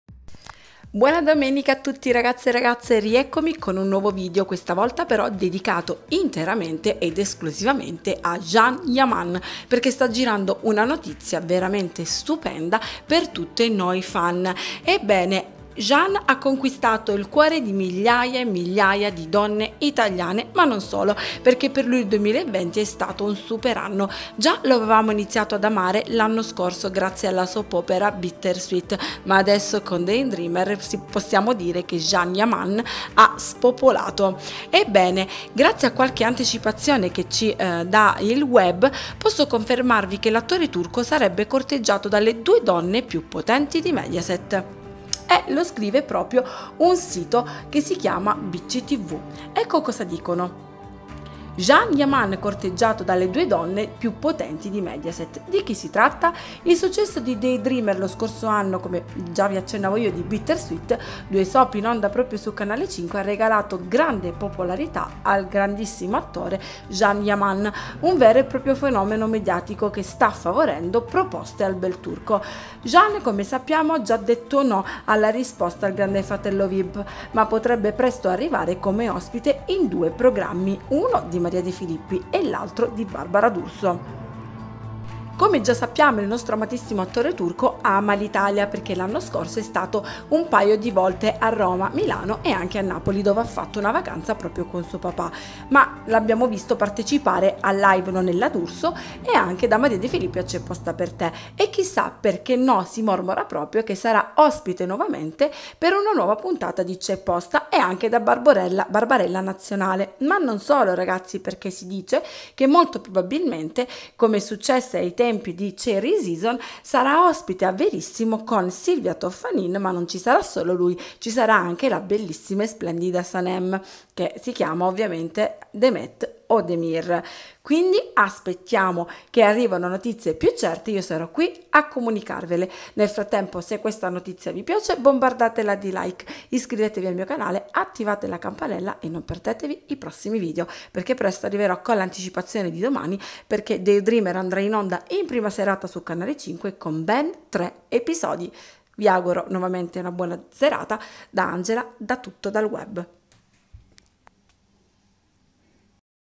Can Yaman Desiderato da tutti!! Presto in Italia,in 2 programmi Mediaset.
Gianni yaman l'uomo più popolare in questo momento in Italia ben presto forse in autunno non lo vedremo in 2 programmi Mediaset